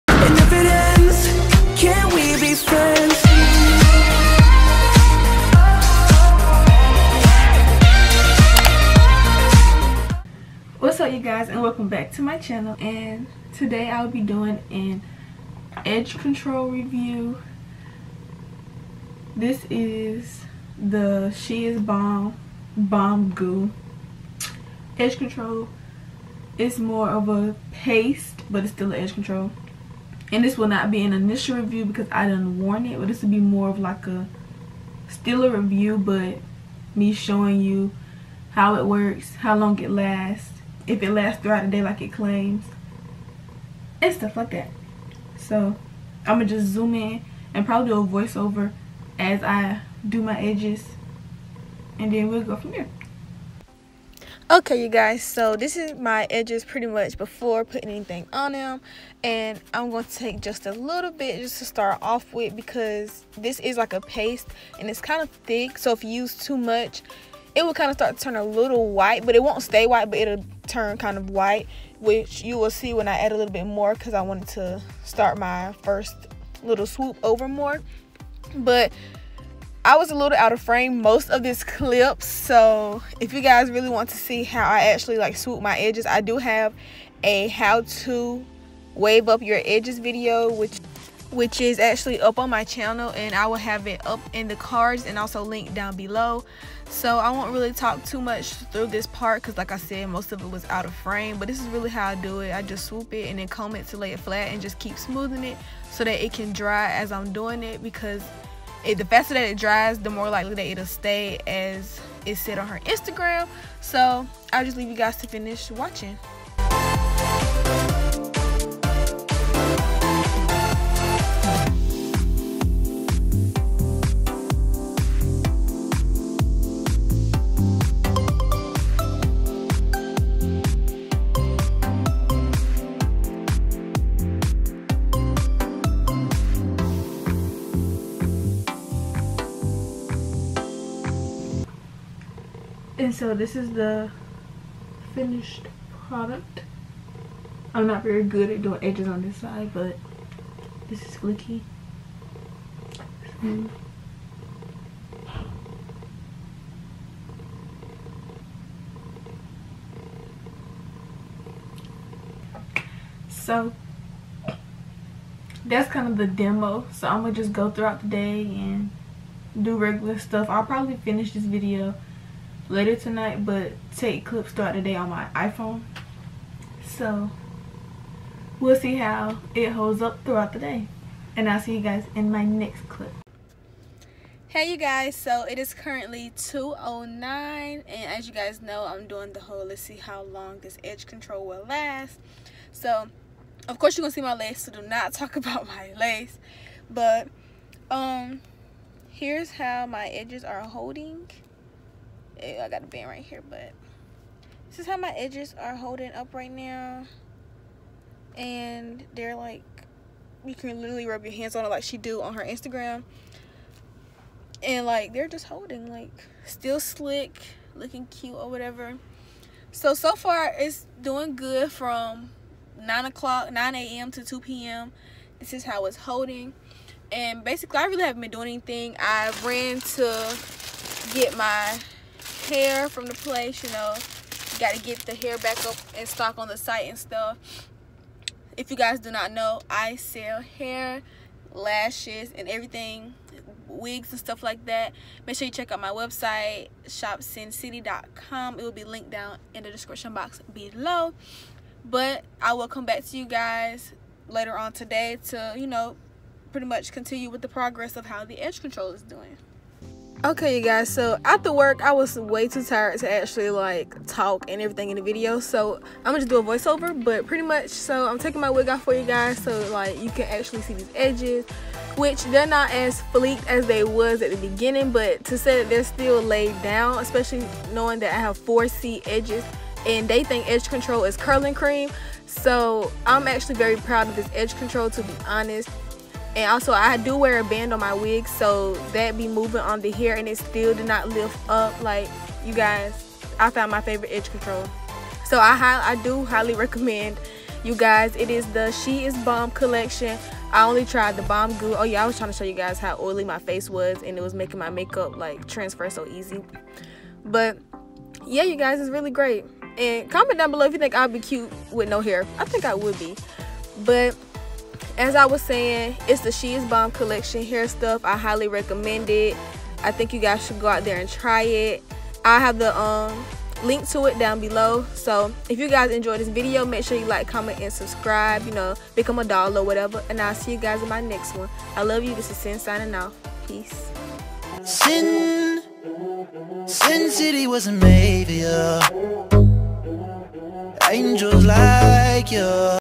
SHE IS BOMB COLLECTION BOMB GOO DEMO & REVIEW!!! | SIMPLY CINN
Hey Dolls💕 in today’s video i will be doing a demo & review of the She is bomb collection bomb goo!

Thank you dolls for 2,488 subscribers! I truly appreciate you dolls 😘💖

▶️FTC: This Video is not sponsored. My opinions are honest and true.

Give $2 Get $2 with my Mercari Code:
CTMFPF
Shop my closet: @_cinn_